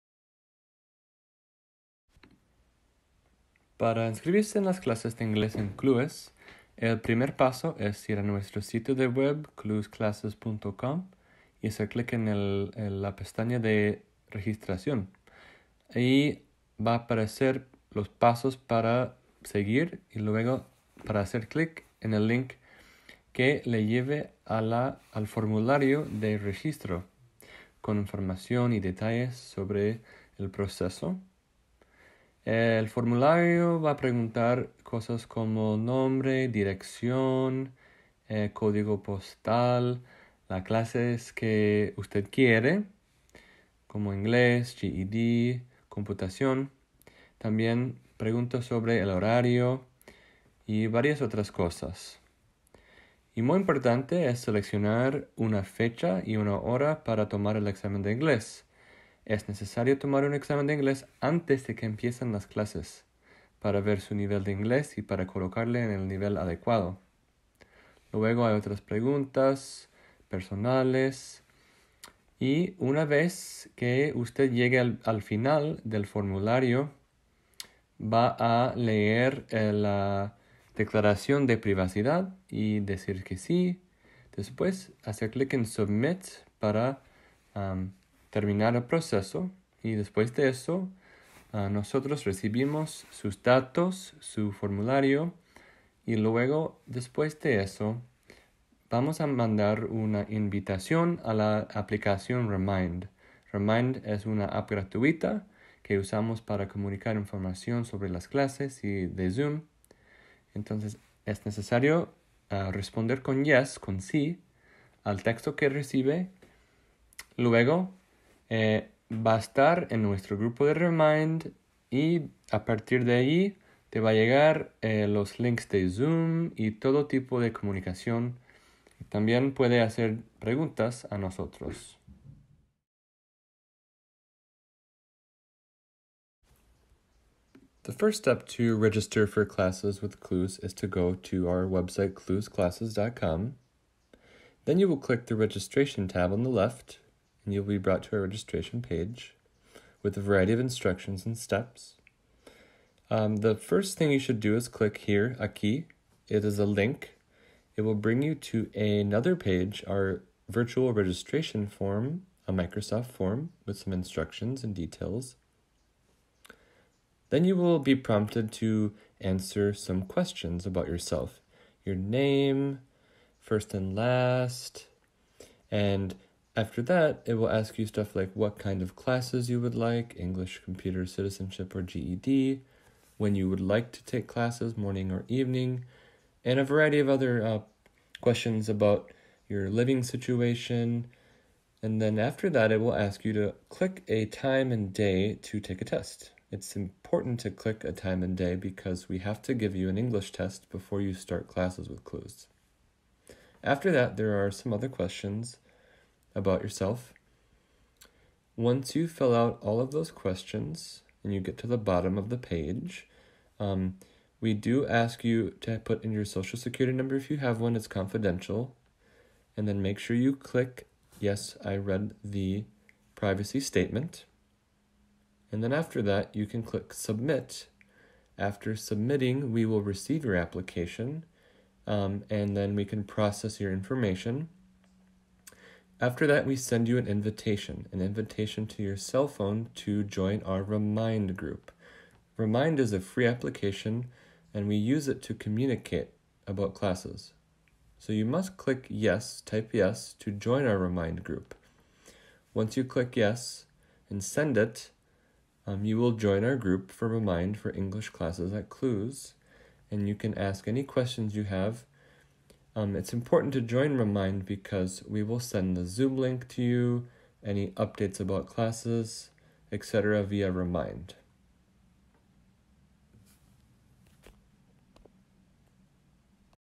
Cómo inscribirse en las clases de inglés de CLUES | How to register for English classes at CLUES
¡Aprendan cómo inscribirse en las clases de inglés en línea de CLUES!
Las clases son 100% gratuitas para todos los adultos que viven en Minnesota.
Learn how to register for CLUES ESL classes online! Classes are 100% free and open to all adults living in Minnesota.
See you in class! ¡Nos vemos en clase!